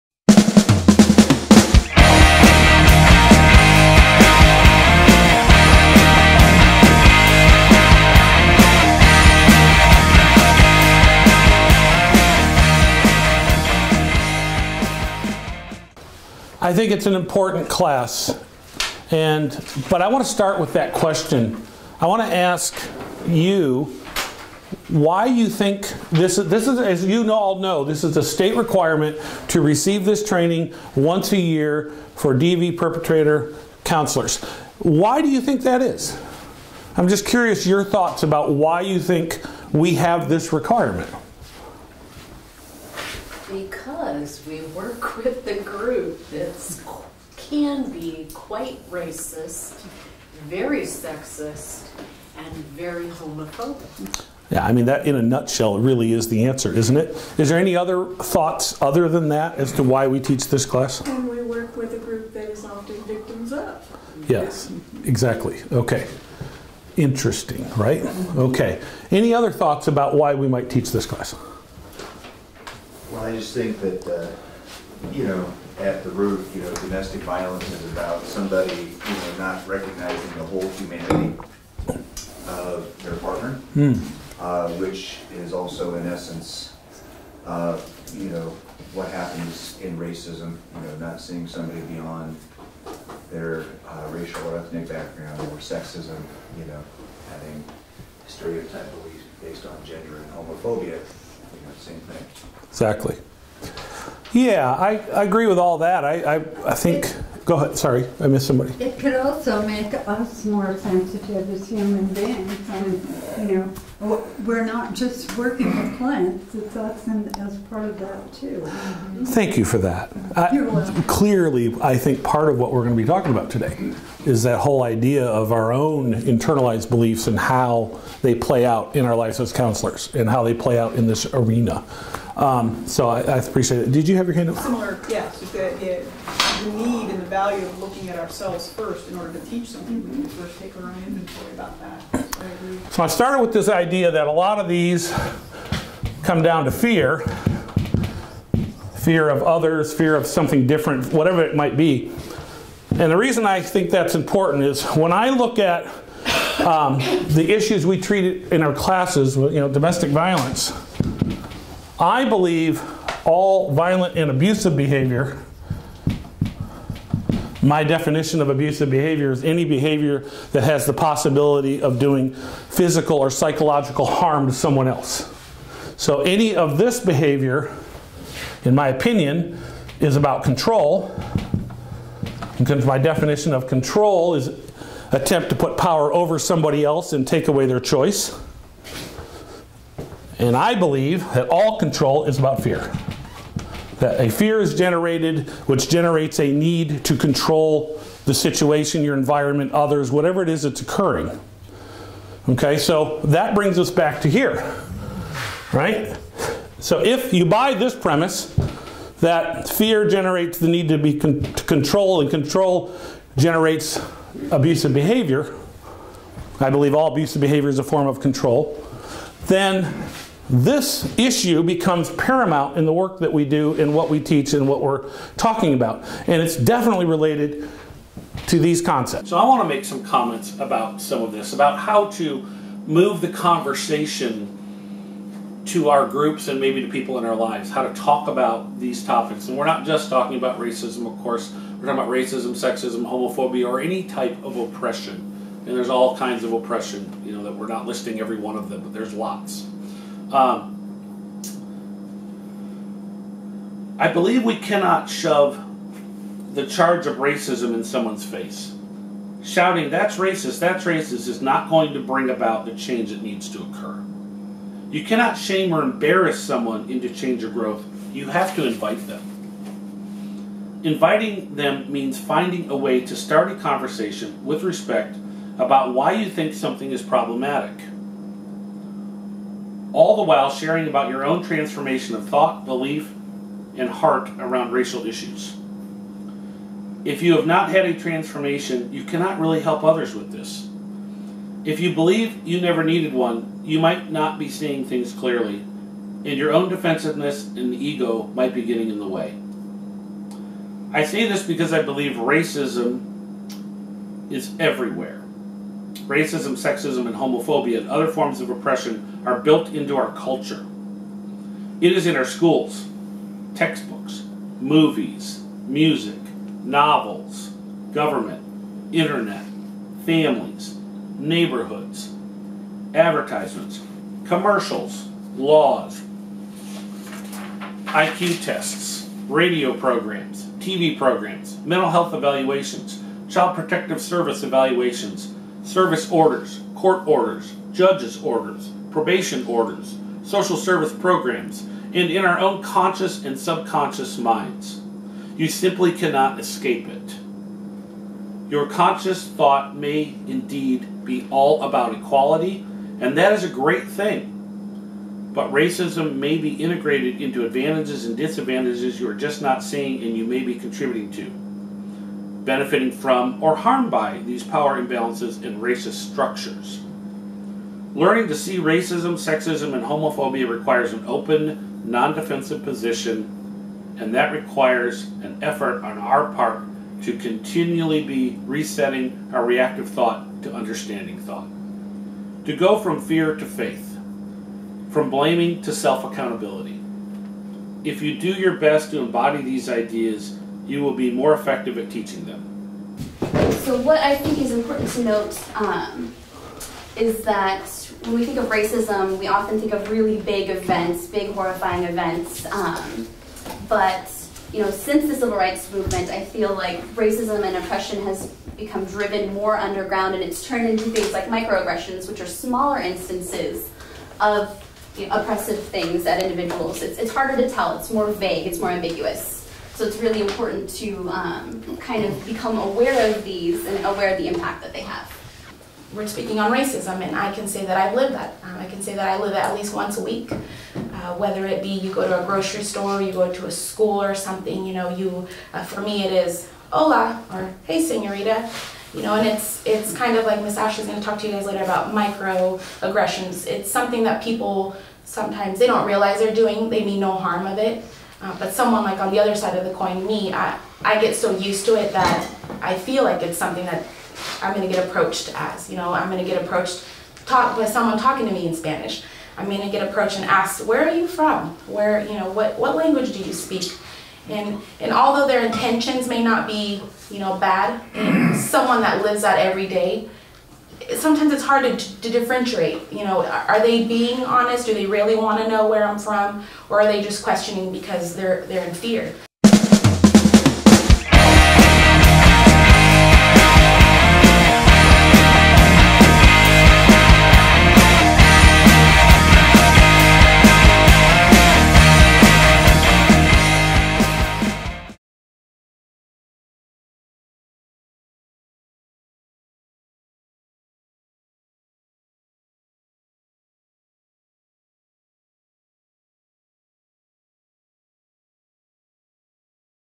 Racism, Sexism, Homophobia, and Other Forms of Oppression
This is a WA State required class for Domestic Violence Treatment Providers. You can contact A Better Way Counseling Service at if you want to purchase the complete 6 hour video ($90.00). You will receive 6 CEU's upon completion.